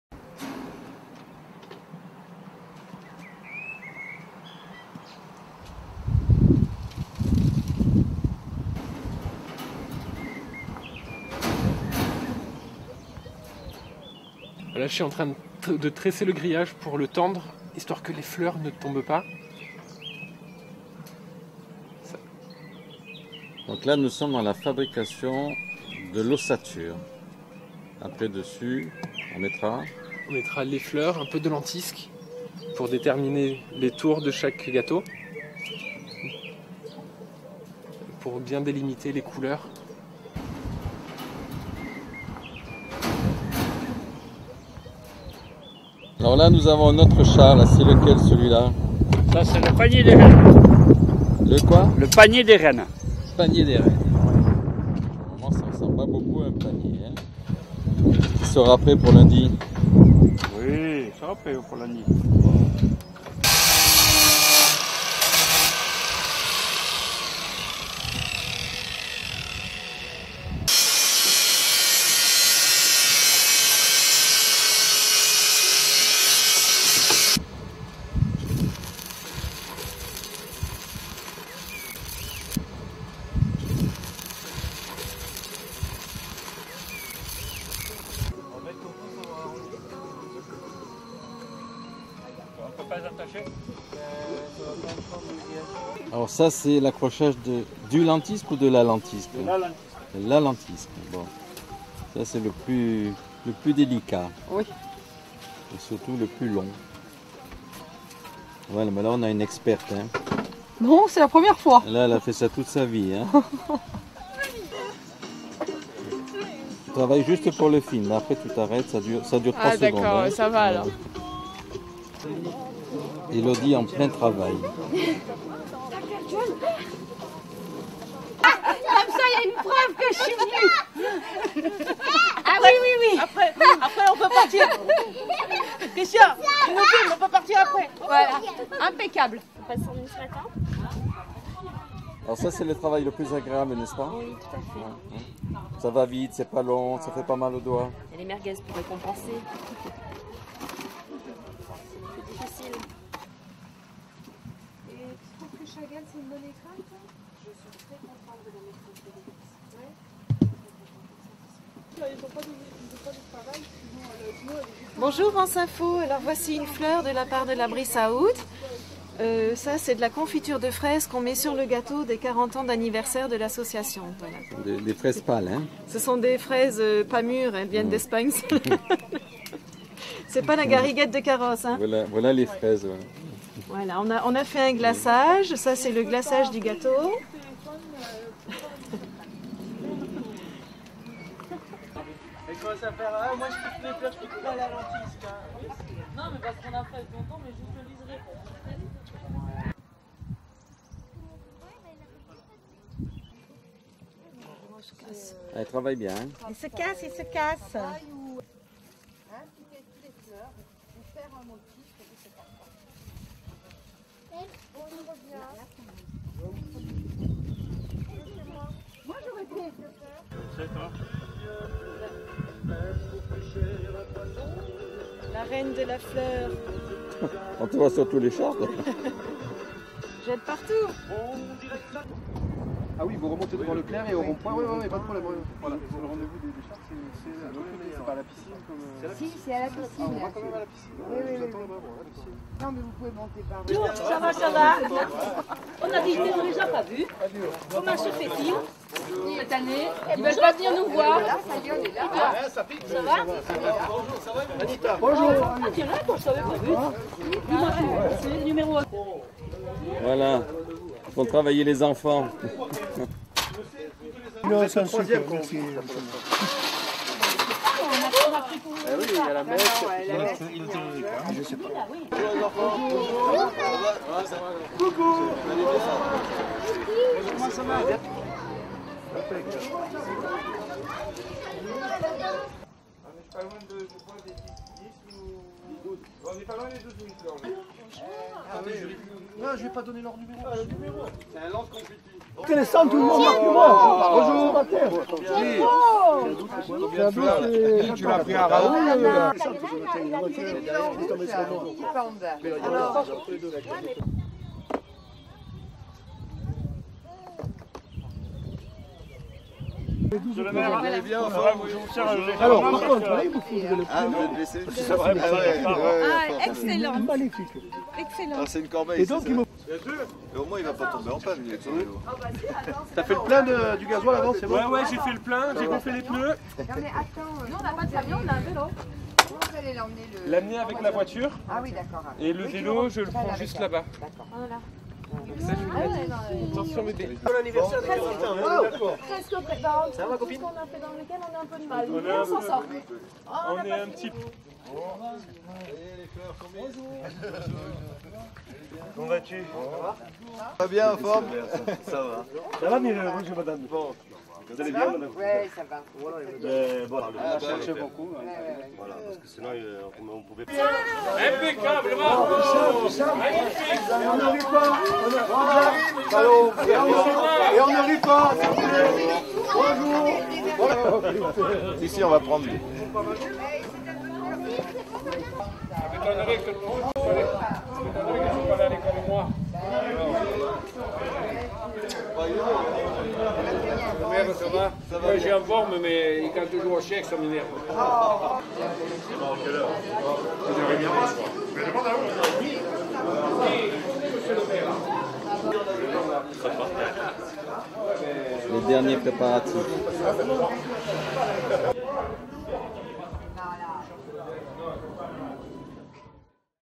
2011 construction chars Pâques c
Chaque année à Vence a lieu le corso fleuri au moment ds fêtes de Pâques. Grands et petits sont ravis lors du corso et de la bataille de fleurs.
Quelques jours avant de nombreux bénévoles mettent la main à la pâte pour fabriquer les chars...
Voici un reportage de 2011.